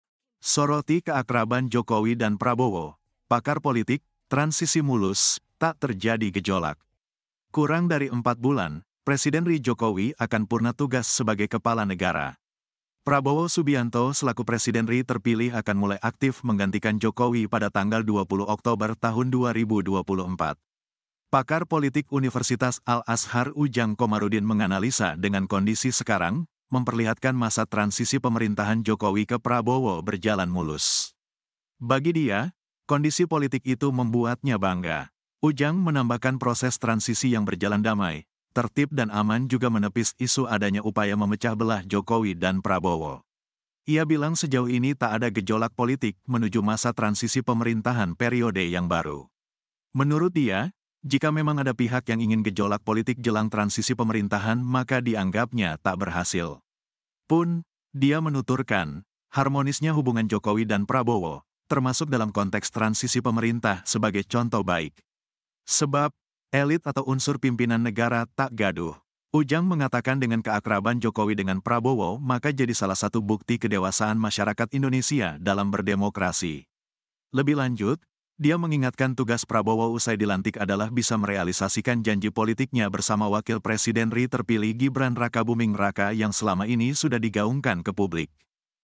Soroti Keakraban Jokowi dan Prabowo, Pakar Politik; Transisi Mulus, Tak Terjadi Gejolak
Kurang dari empat bulan, Presiden RI Jokowi akan purna tugas sebagai kepala negara.